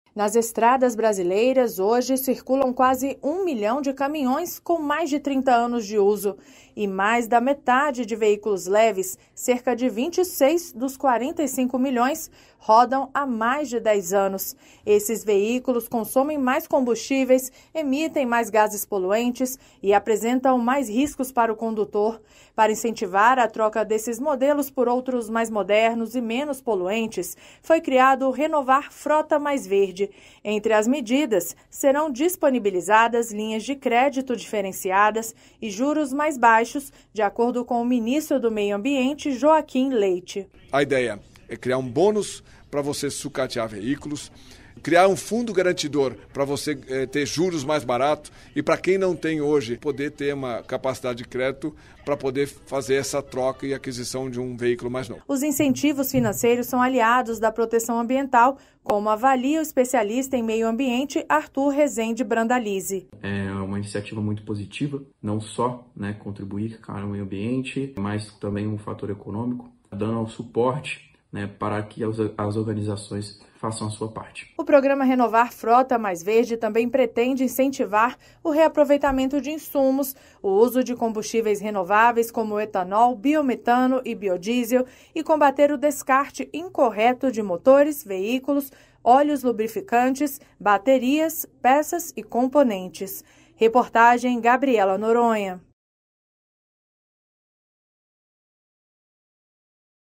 21/11/2022 – RENOVAR FROTA + VERDE ALIA CRÉDITO A PROTEÇÃO AMBIENTAL | Matérias da Voz
Programa do Ministério do Meio Ambiente oferece incentivos financeiros para a compra de veículos mais modernos, considerados menos poluentes e mais sustentáveis.